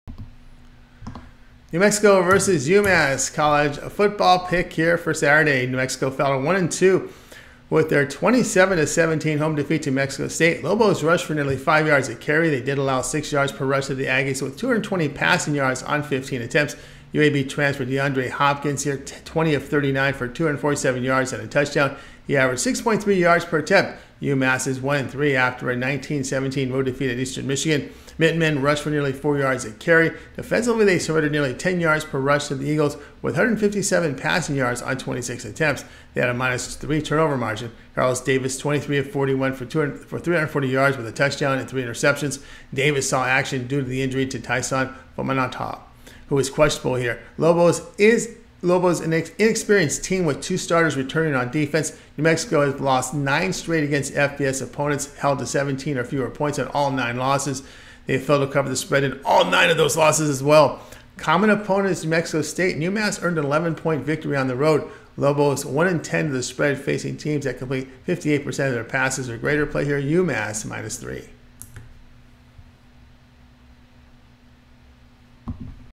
New Mexico vs. Massachusetts 9/23/2023 Week 4 FREE College Football Picks and Prediction for Today
9/23/2023 New Mexico vs. Massachusetts FREE College Football Picks and Prediction on NCAAF Betting Tips for Today